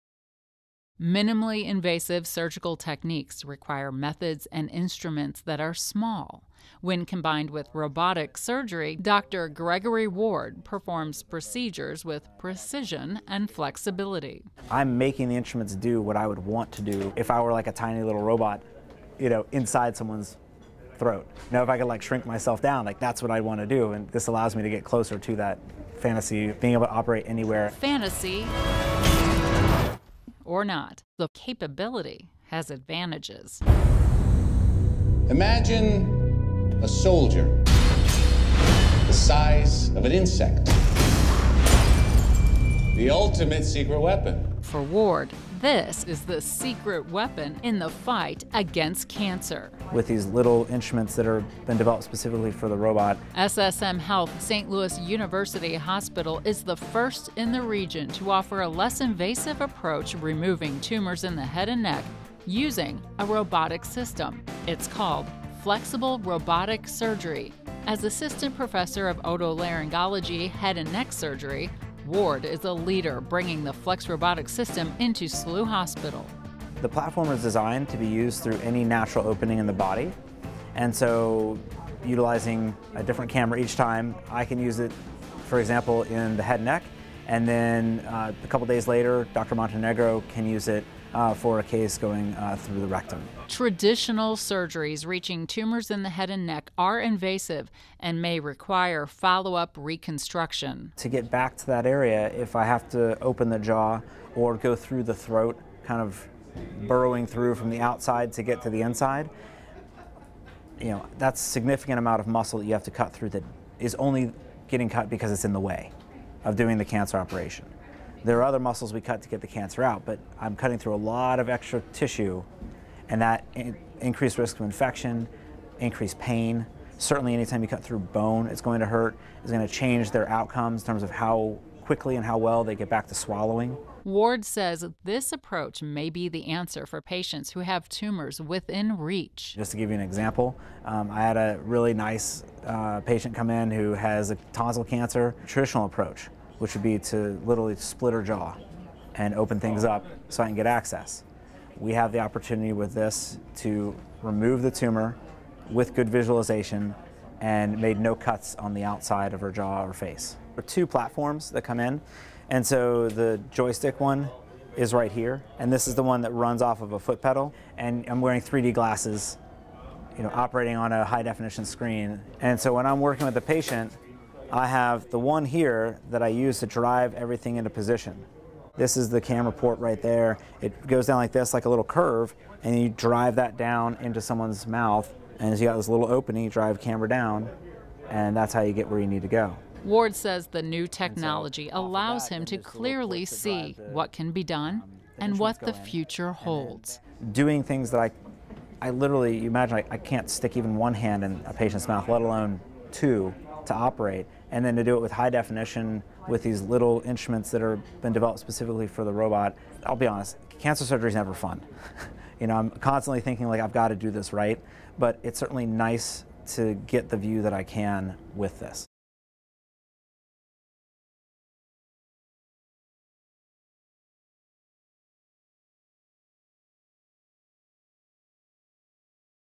Saint Louis University Hospital is First in Region to Offer Flexible Robotic Surgery System
Minimally invasive surgical techniques require instruments that are small. Combined with robotic surgery, Gregory Ward, M.D., performs innovative procedures at Saint Louis University Hospital. The Flex Robotic System is essentially robot-assisted surgery. “I’m making the instruments do what I would want to do if I were a tiny robot inside someone’s throat,” said Dr. Ward. “The platform is designed to be used through any natural opening in the body." For patients with laryngeal cancers, for example, Ward can now insert a single, fully flexible robotic arm into the mouth and control the machine with a joystick-like device and foot pedal. Ward said the system’s HD cameras and 3D glasses allows him to operate using a high-definition screen.